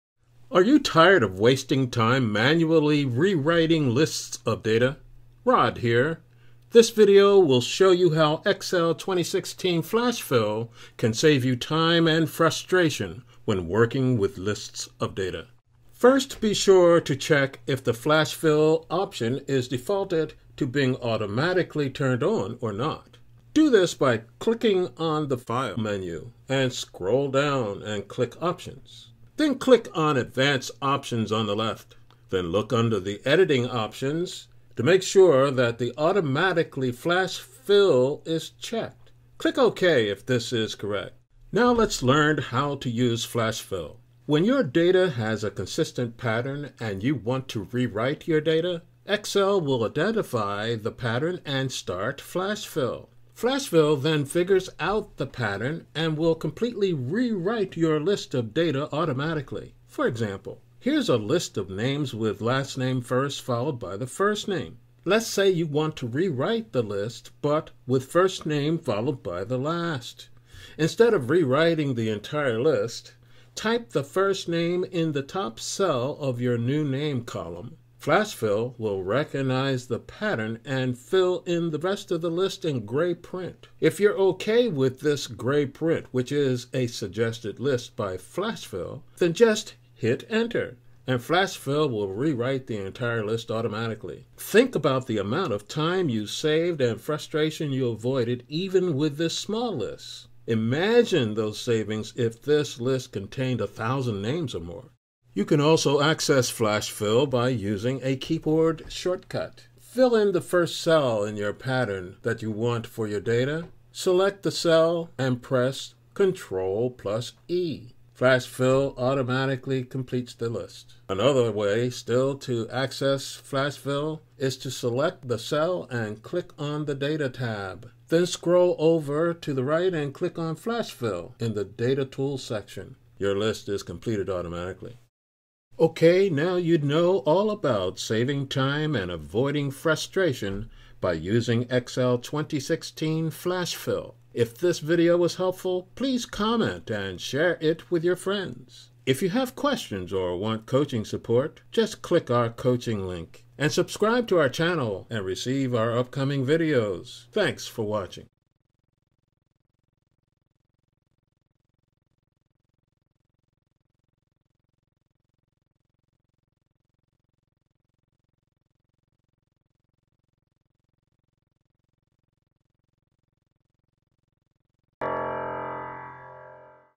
How to Use Flash Fill | Excel
This video shows you how Use Flash Fill Lists. Burn to learn focuses on a simple way to solve this problem. We show you
how to access it by using a keyboard shortcut and then save time and frustration . Learning how to use Flash Fill is what this video is